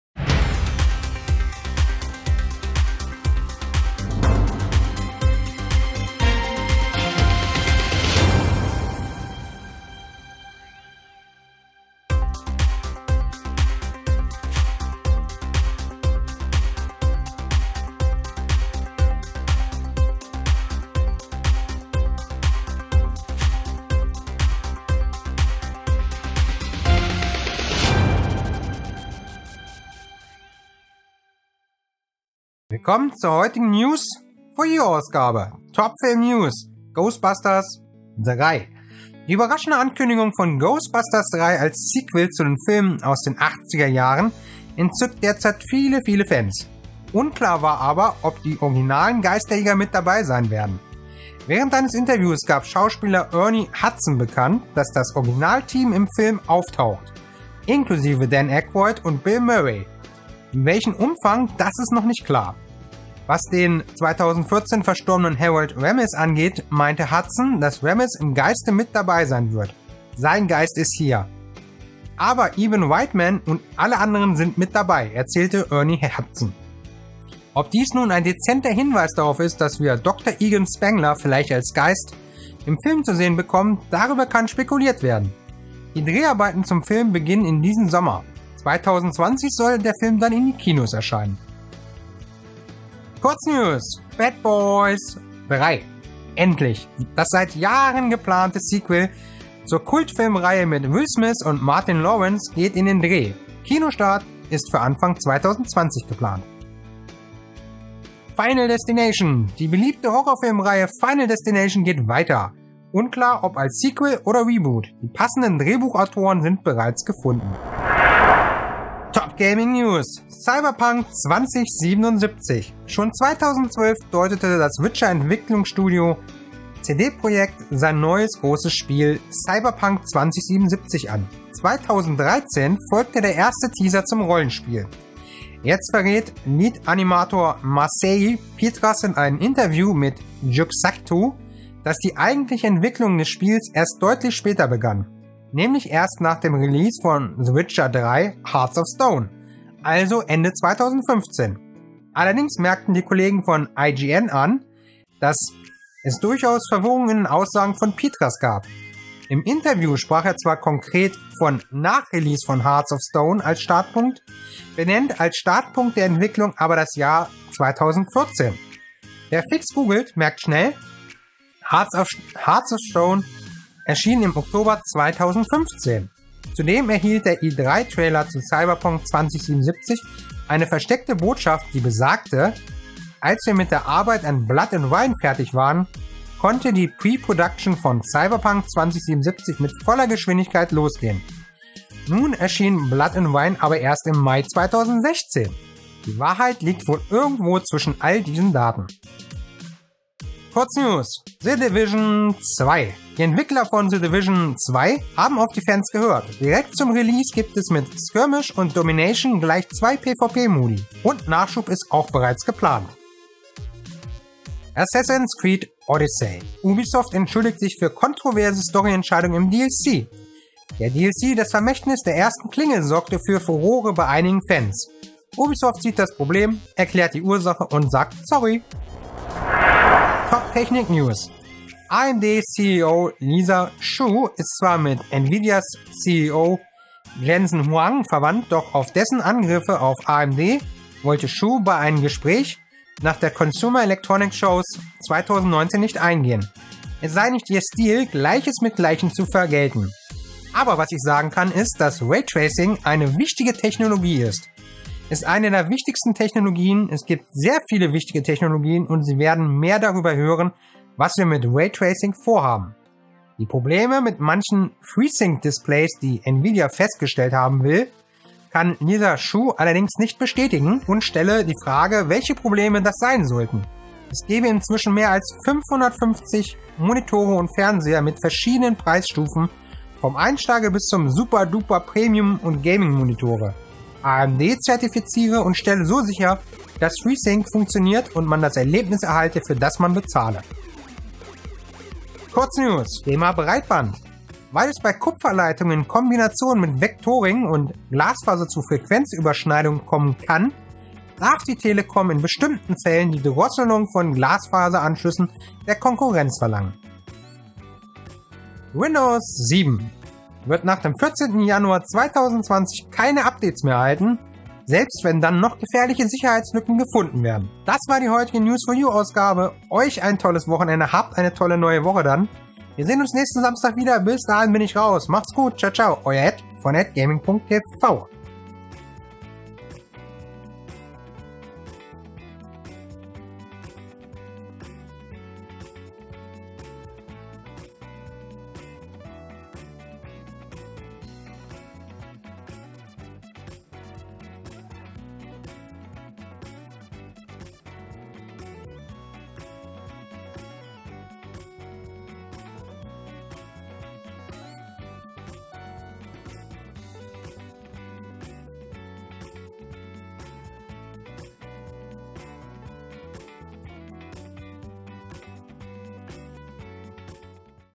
* NEWS 4 YOU * 2019 #003/ Heute: GHOSTBUSTERS 3/CYBERPUNK 2077/AMD CEO SU [DEUTSCH] HD
▼▼▼▼ Möchte Ausgeklappt werden ▼▼▼▼

Alle News werden hier verlinkt zum Nachlesen:
Alle Games die in den News vorkommen, gibt es hier günstiger zu kaufen:
Ihr unterstützt mich damit, ohne Mehrkosten für euch!

Willkommen zu einer neuen Ausgabe von *NEWS 4 YOU* 2019

Heute mit: Ghostbusters 3, Bad Boys 3, Final Destination, Cyberpunk 2077, The Division 2, Assassin's Creed: Odyssey, AMD-CEO Lisa Su, Breitband, Windows 7